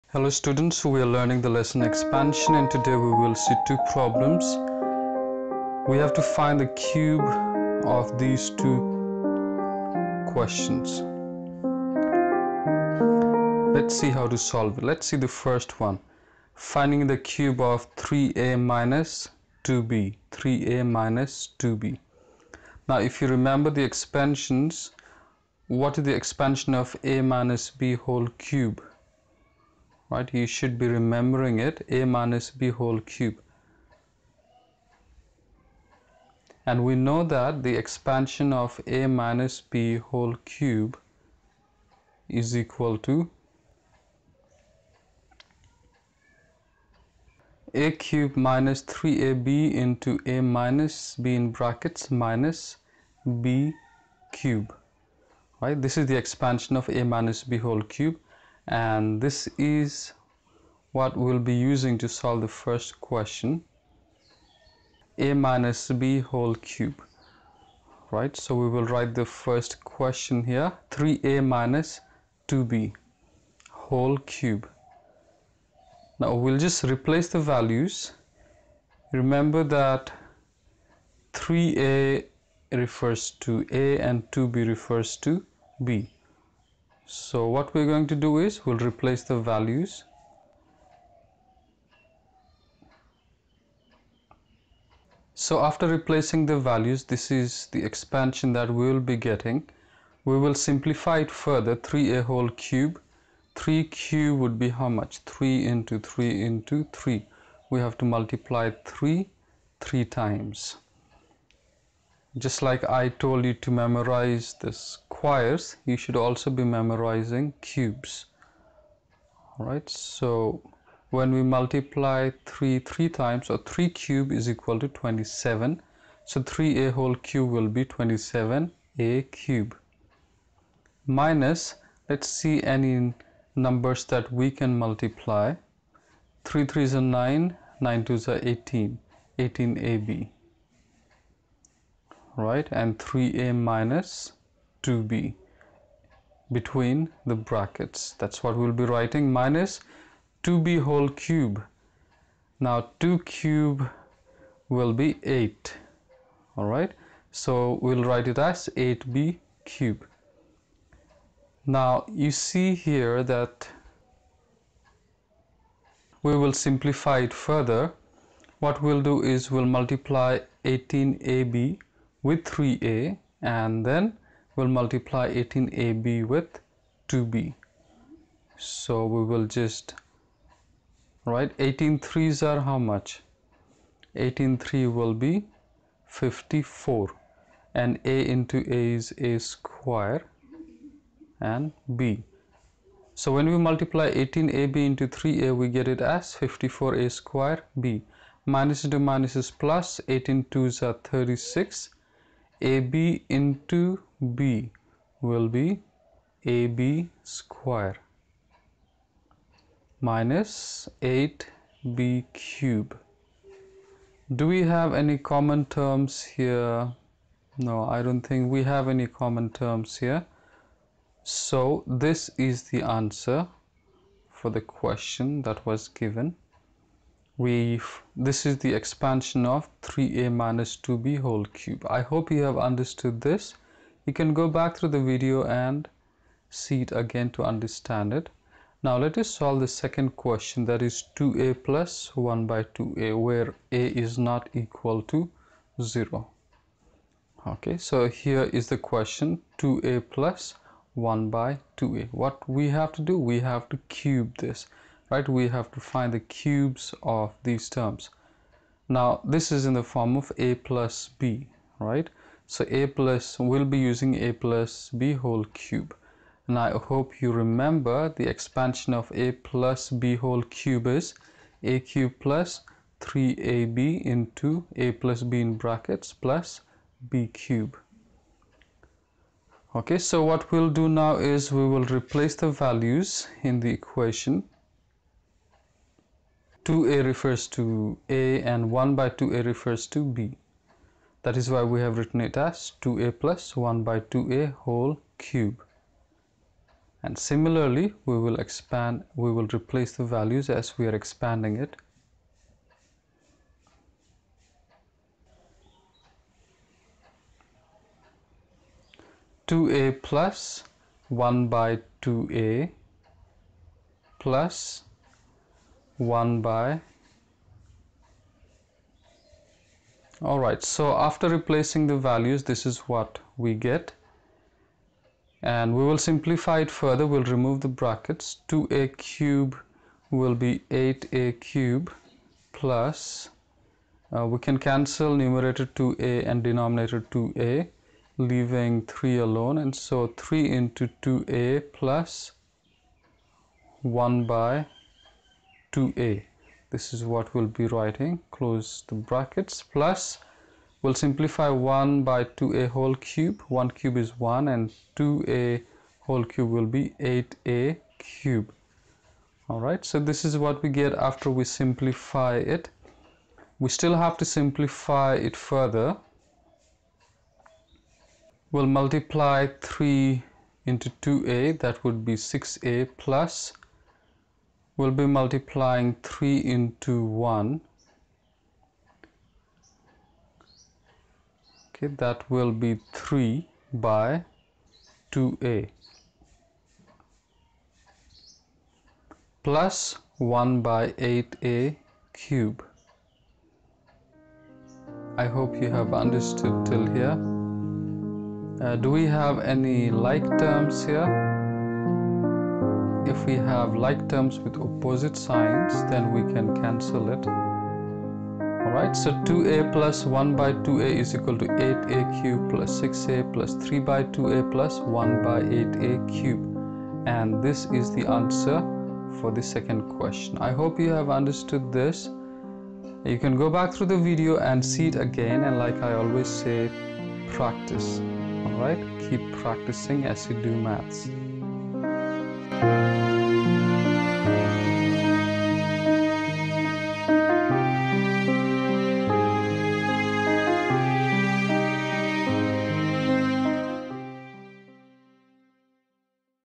Finding Cube || Class 8 || Algebra
Understanding how to find cube in expansion (Algebra)